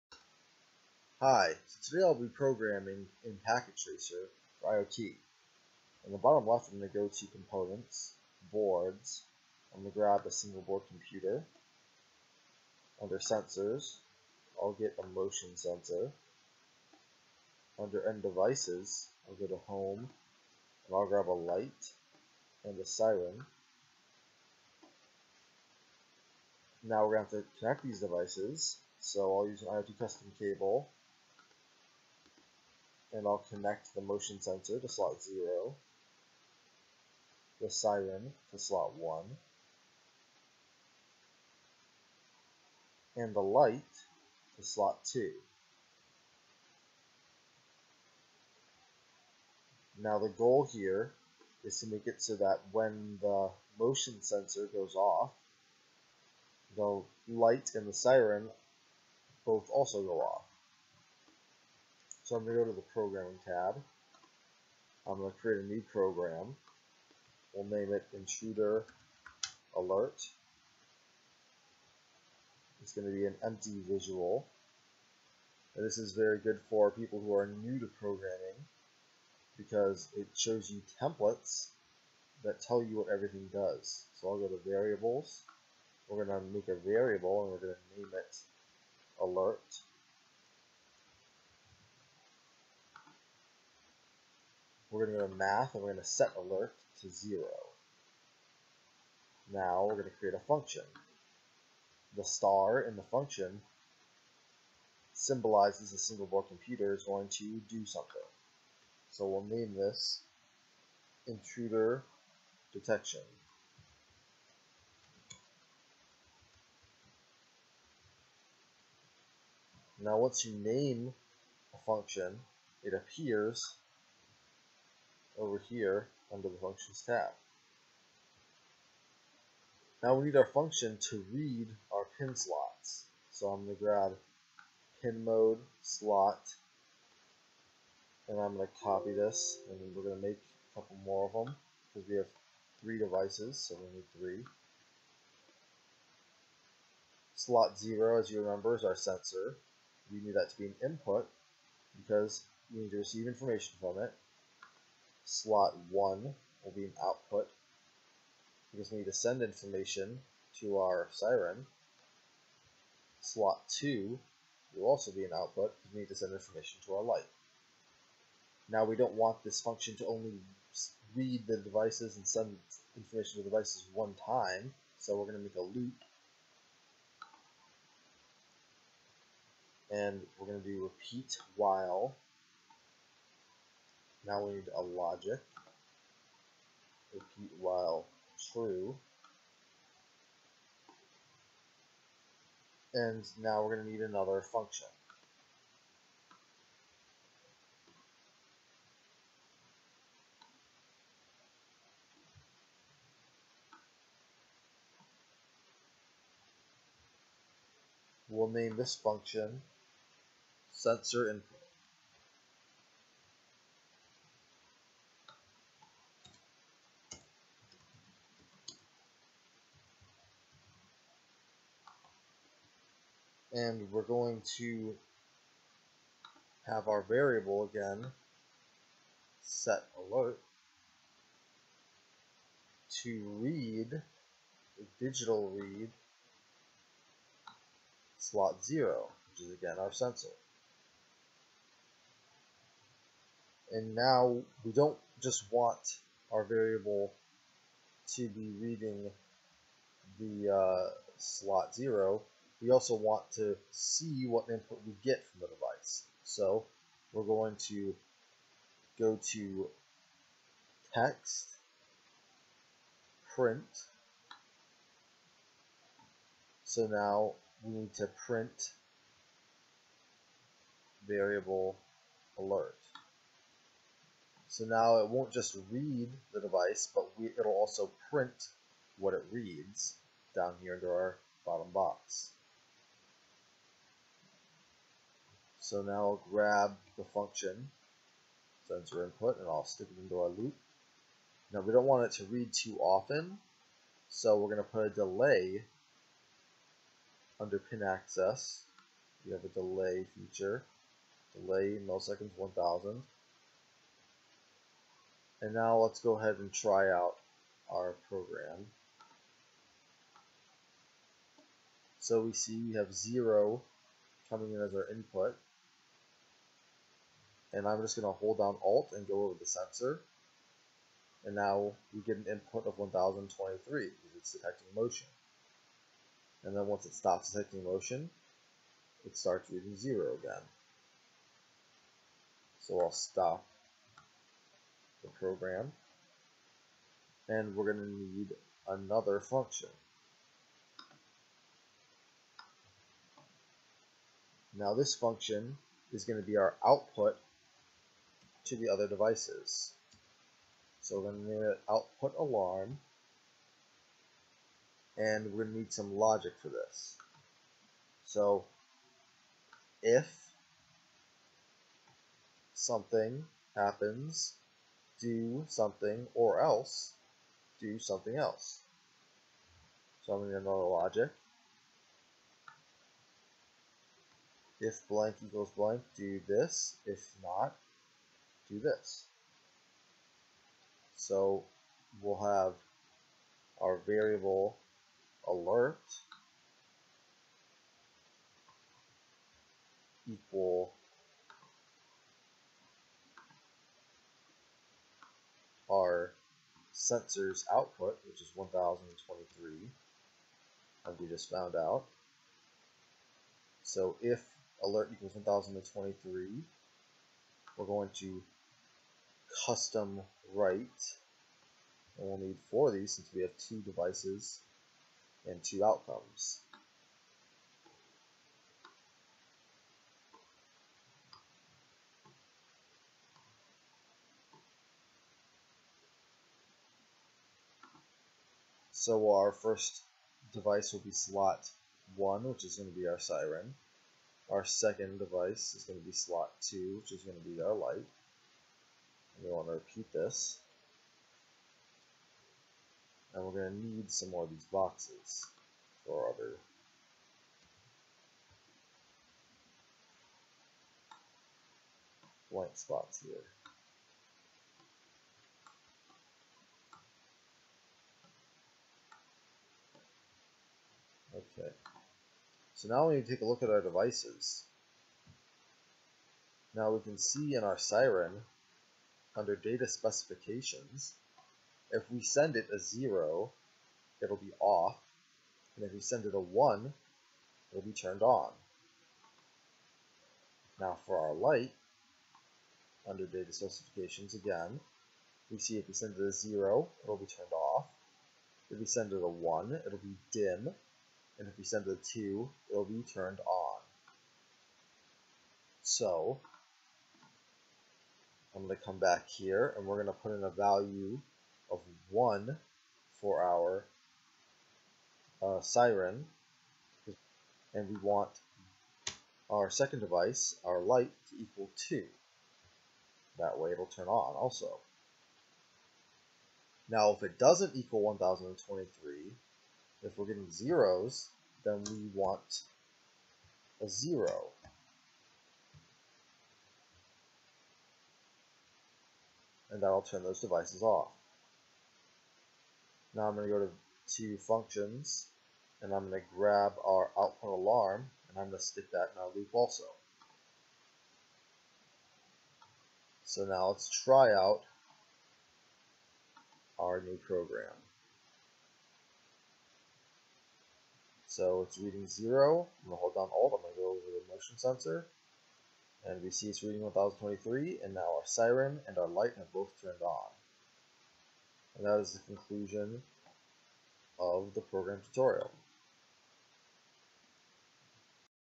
Packet Tracer IoT Tutorial video 5 Basic Programming
In this video I show how to create a basic program within Packet Tracer for integration into an IoT network.